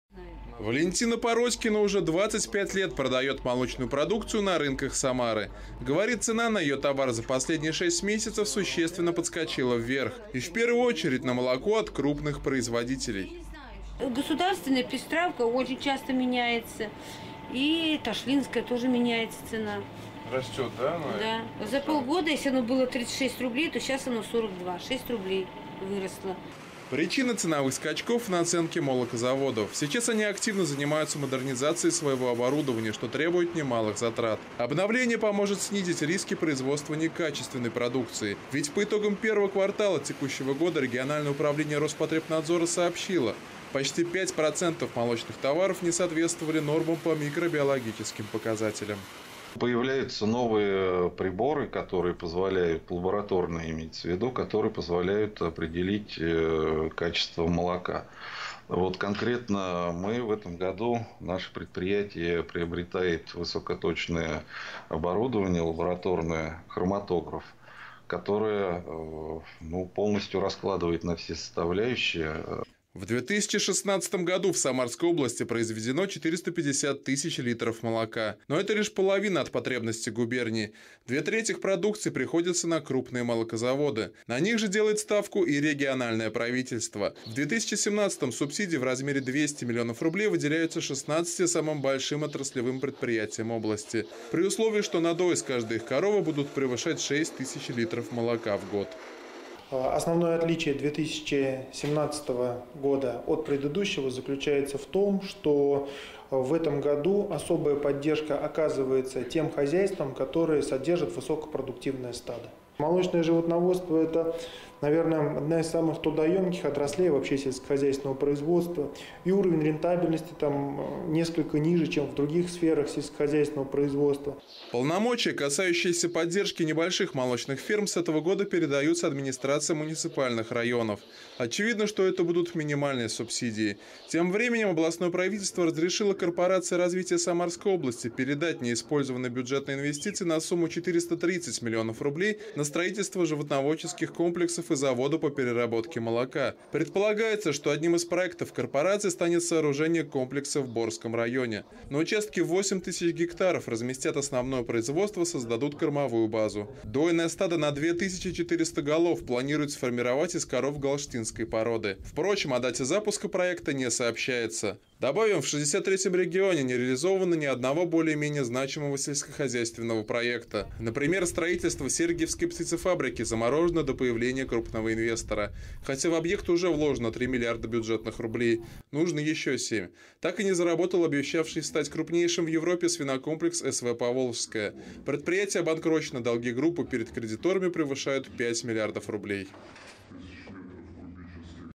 Дефицит молока
Самарская область обеспечивает себя молоком лишь на 55%. Недостающая доля продукции завозится из соседних регионов. При этом население губернии потребляет молочных товаров меньше, чем положено по медицинской норме, установленной федеральным Минздравом, отвечающей современным требованиям здорового питания. Вместо необходимых 300 килограммов молока в год, на одного человека приходится всего 260. Одновременно снижается поддержка небольших хозяйств, развитие и укрупнение которых могло бы повлиять на ситуацию. Сможет ли Самарская область преодолеть дефицит молока и что будет с ценами на продукты, выяснял Леонид Кошман.